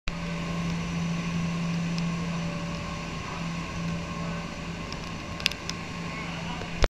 Kanakis
Olive oil pressing, no chemicals used.  Filtered one with paper.